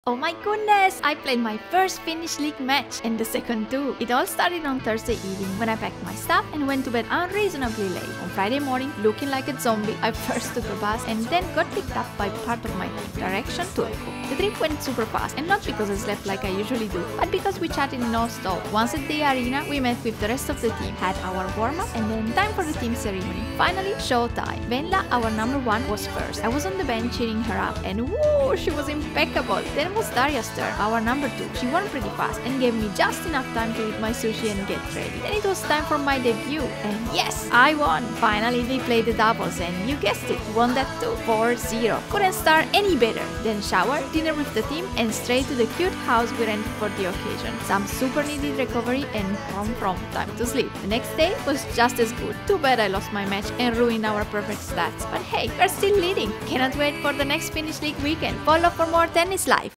My Finnish Tennis League debut was a blast! 💃
After a few weekends as bench player, this past one I played my first Finnish League match! And the second one too!! How cool was that?! Oh my goodness!! I still cannot believe it happened!! Goal ACHIEVED!

💌 Sign up to my newsletter to join my tennis adventures and learn with me!